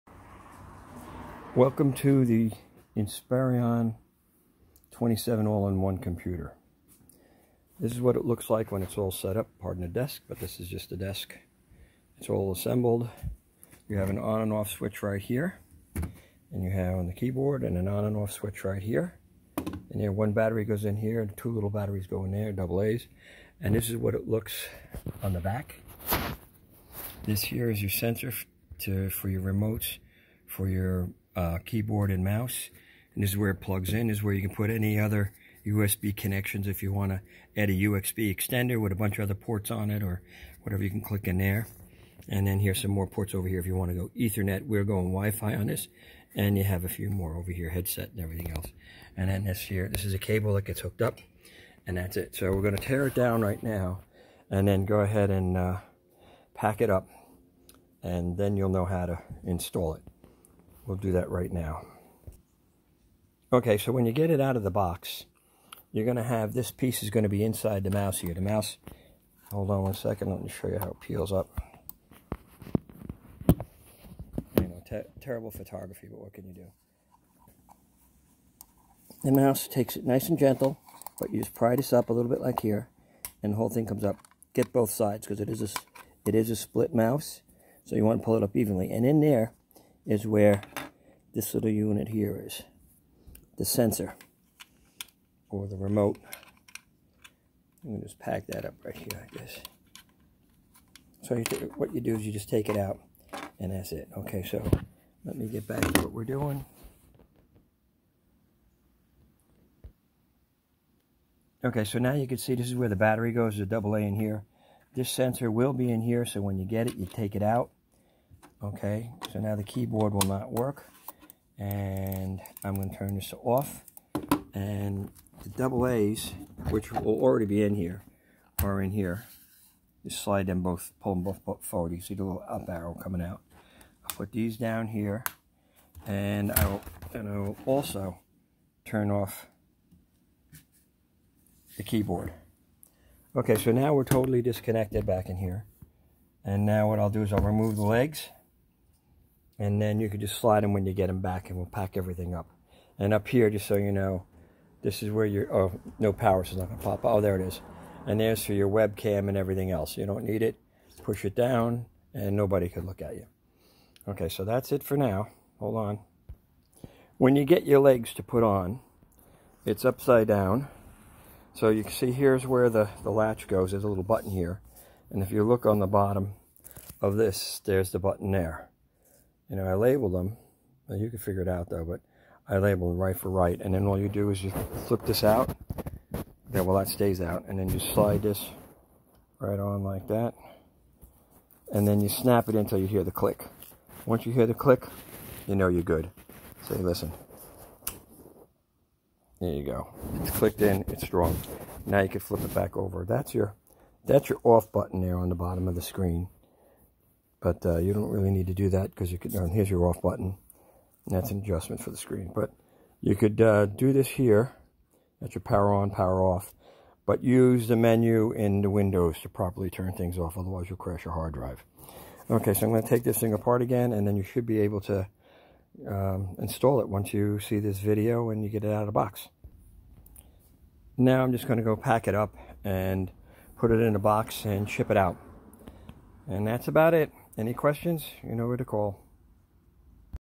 Dell Inspiron 27 inch all in one installation
Dell Inspiron 27 inch all in one installation or assembly

Dell inspiron assembly for 27 inch all in one pc

how to assemble the Dell Inspiron 27" All in One Pc